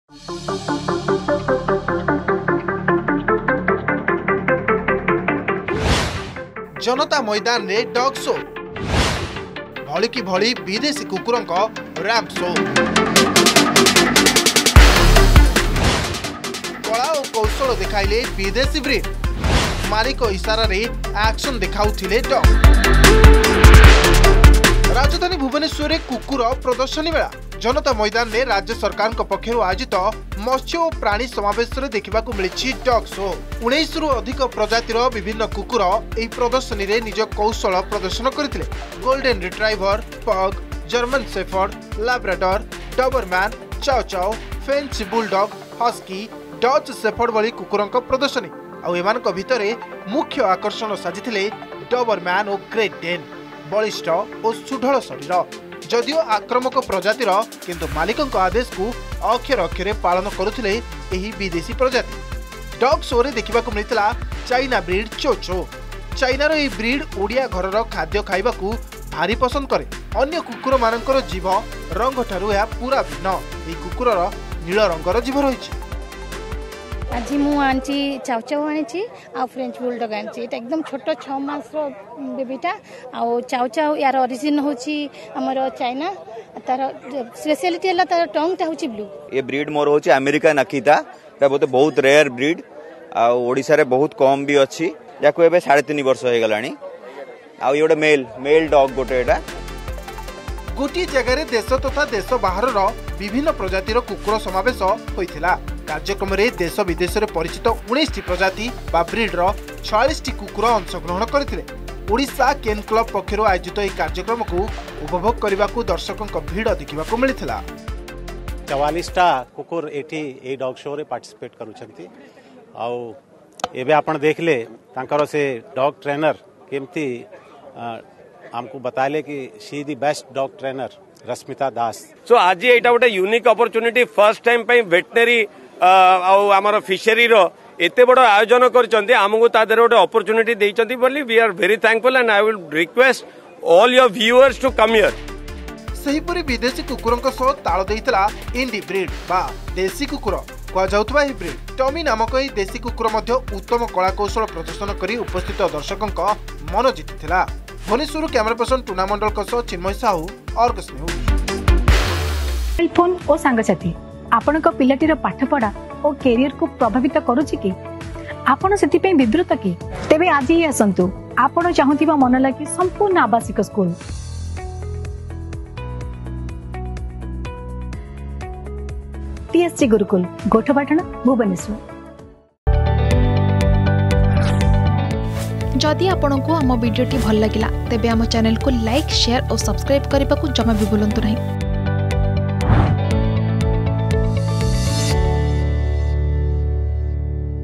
ଭୁବନେଶ୍ୱର ଜନତା ମଇଦାନରେ କୁକୁର ପ୍ରଦର୍ଶନୀ || Dog Show || Matsya- Pranee Samavesh Odisha
ଭୁବନେଶ୍ୱର ଜନତା ମଇଦାନରେ କୁକୁର ପ୍ରଦର୍ଶନୀ। ରାଜ୍ୟ ସରକାରଙ୍କ ପକ୍ଷରୁ ଆୟୋଜିତ ମତ୍ସ୍ୟ ଓ ପ୍ରାଣୀ ସମାବେଶରେ ଦେଖିବାକୁ ମିଳିଛି ଏହି ଡଗ ସୋ। ୧୯ରୁ ଅଧିକ ପ୍ରଜାତିର ବିଭିନ୍ନ କୁକୁର ଏହି ପ୍ରଦର୍ଶନୀରେ ନିଜ କୌଶଳ ପ୍ରଦର୍ଶନ କରିଛନ୍ତି ।